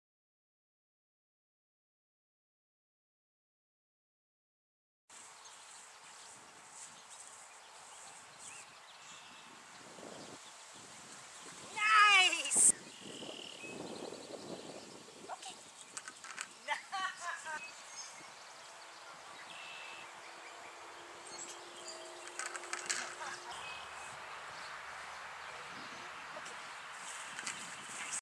2014 05 29 W WP training
Willie sometimes hits the poles in the channel instead of weaving around them.  I decided it was time to go back to my 2x2's which are 24" on center.